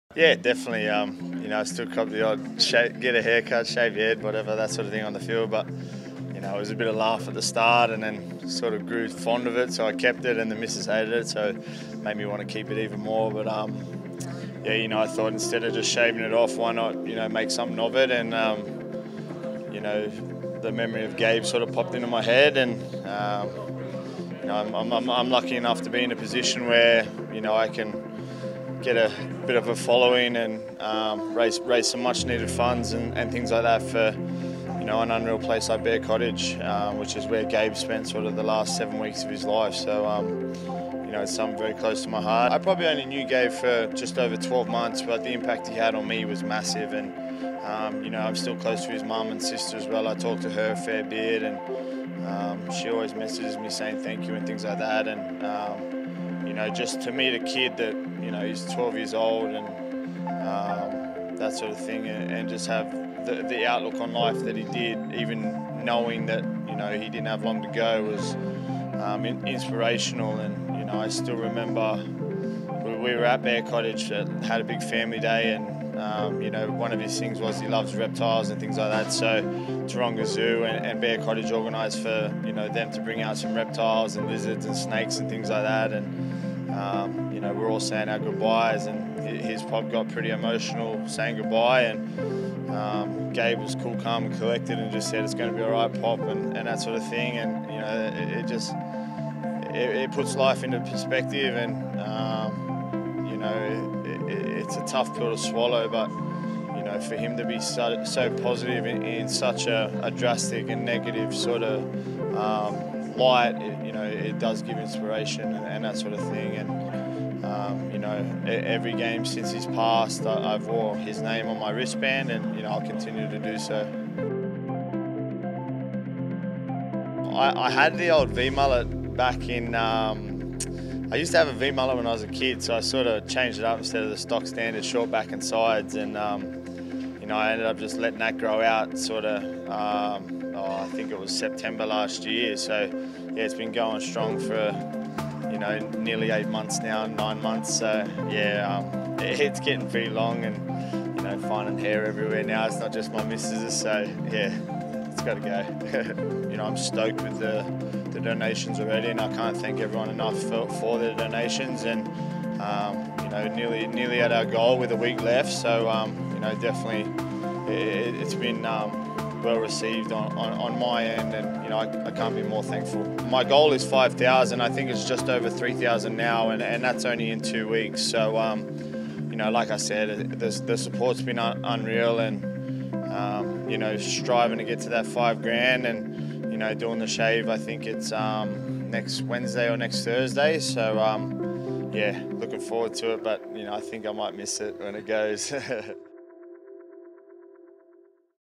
Josh Dugan: The touching story behind his mullet
"I thought instead of shaving it off, why not make something of it"

What a touching story and great gesture this is from Josh Dugan as he explains the emotional reason behind shaving his mullet for charity 👏👏👏